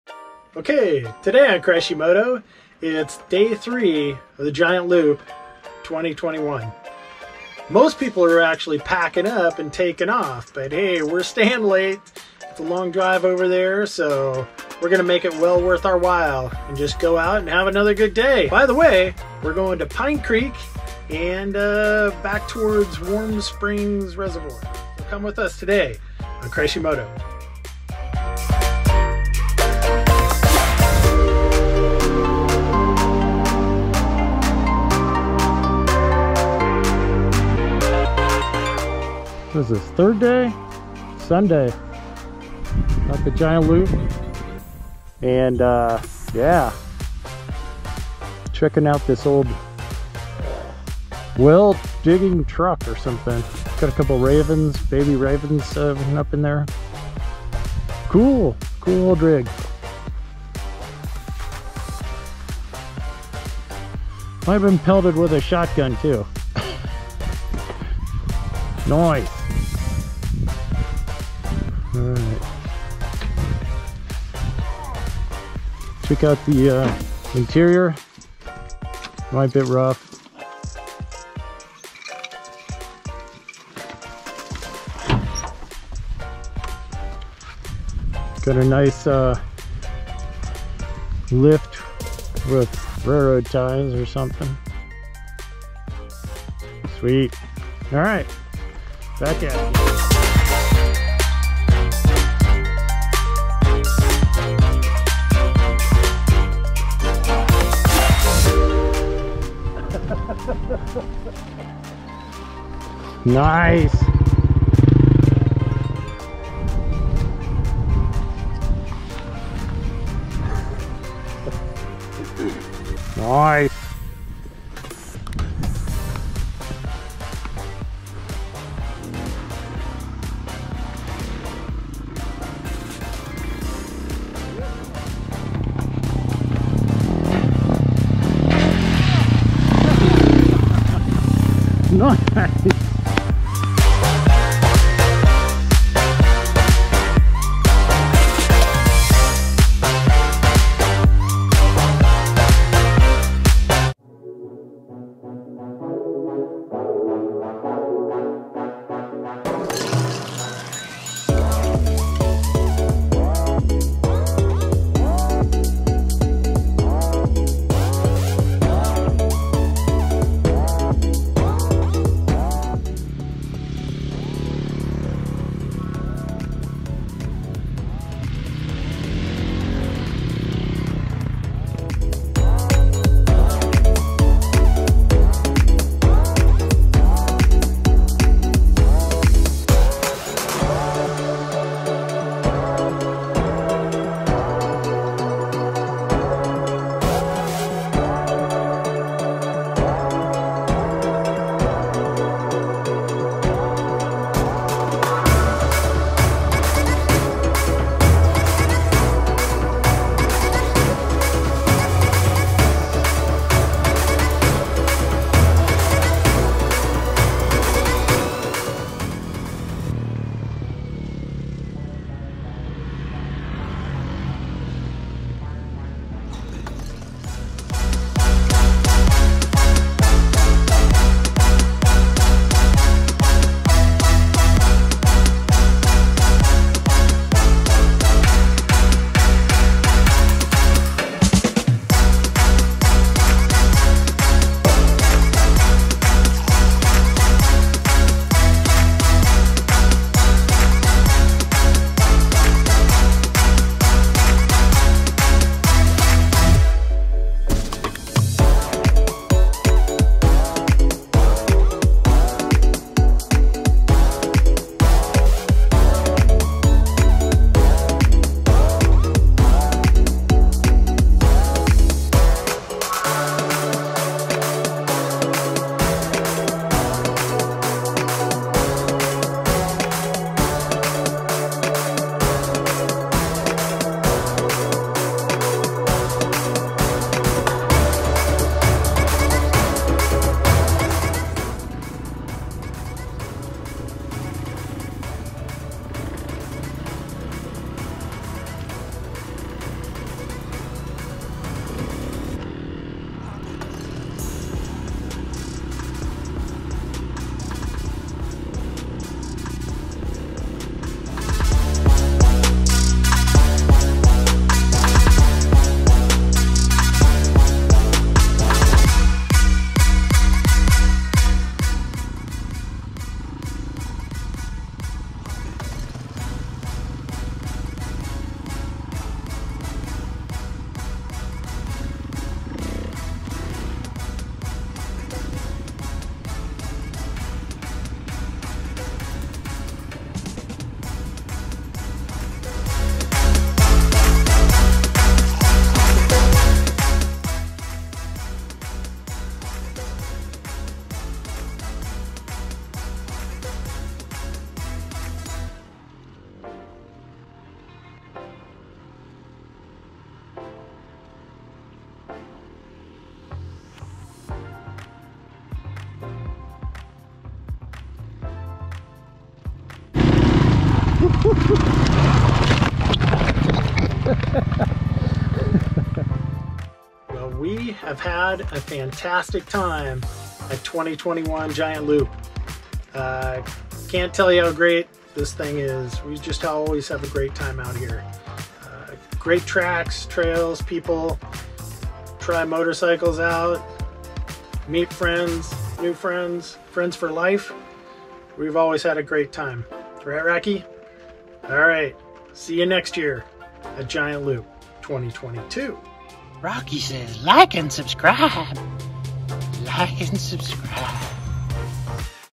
GiantLoop day3 Pine Creek Loop
Today we take a chill day on the two closest trails to camp. This would be a great first trail of the trip or awesome for the big ADV bikes. Pine Creek and Warm Springs Reservoir are great trails for all…….. unless it’s wet, then everything can be a mess.